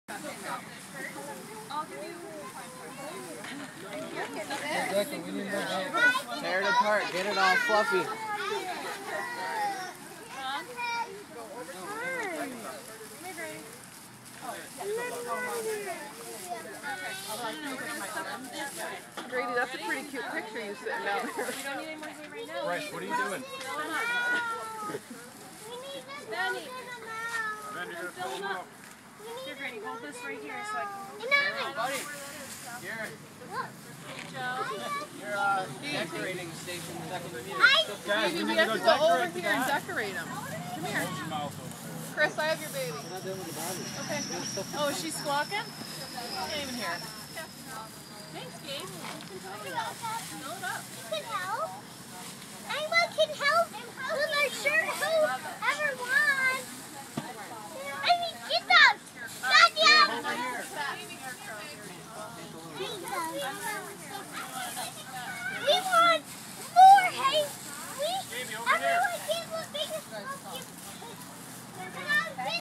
Skaneateles Fall Fest
Families gather Saturday morning to make scare crows at Fall Fest in Skaneateles.  Sarah Jean Condon | The Citizen